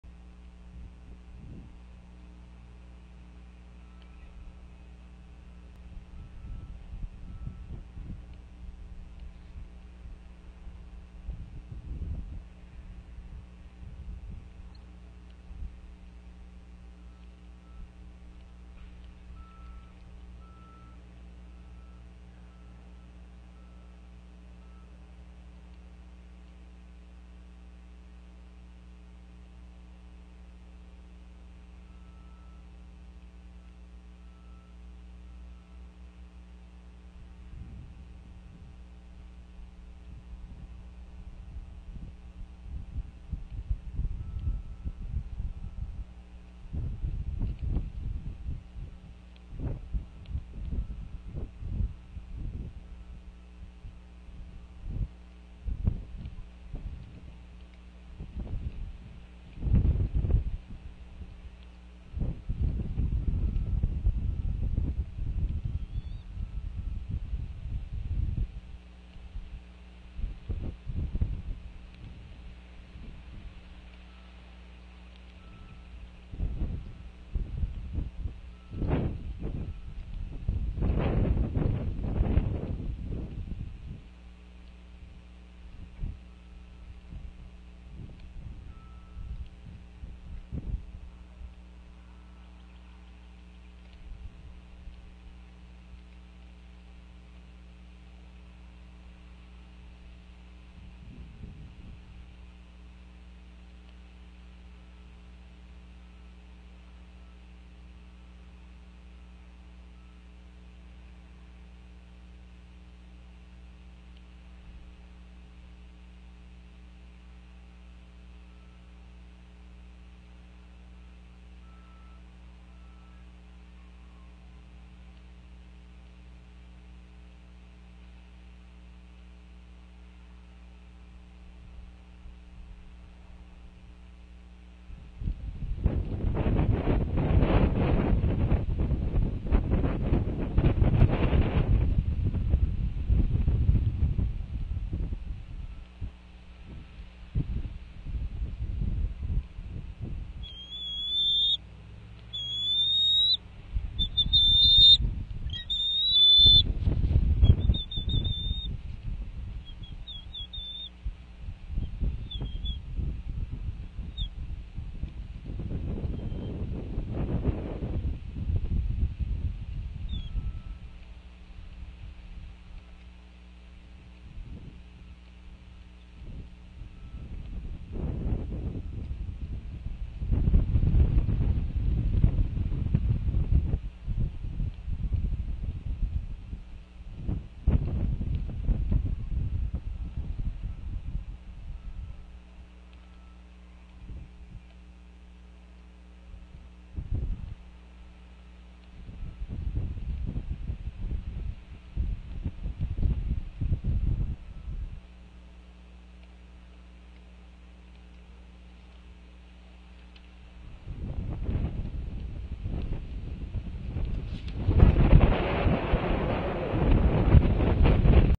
AEF 01-07-2014 Northeast Florida Bald Eagles- LIFE IS GOOD
Time for a long nap after mom fed us. LIFE IS SO FINE IN THE PINE IN THE HAMLET!!